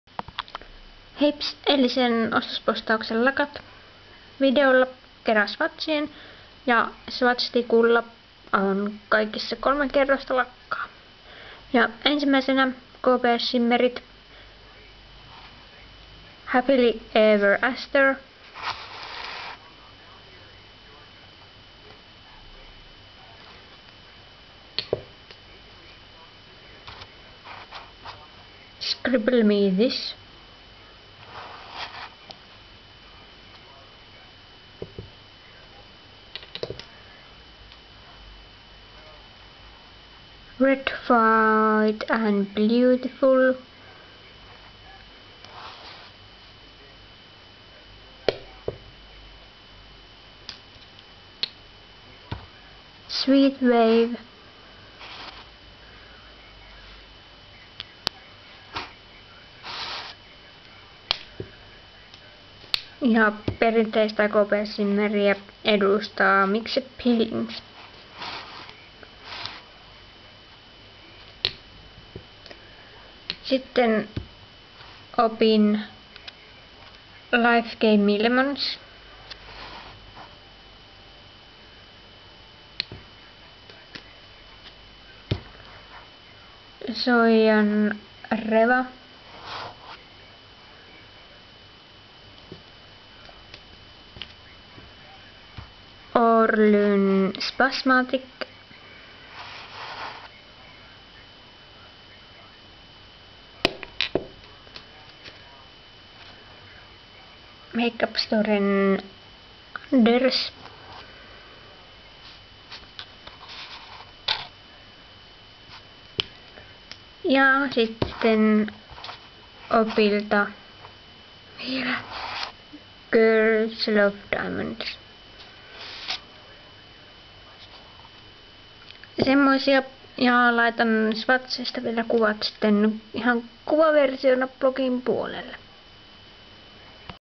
KBShimmer & muut
Lakat videolla + swatchit. Swatcheissa kolme kerrosta lakkaa.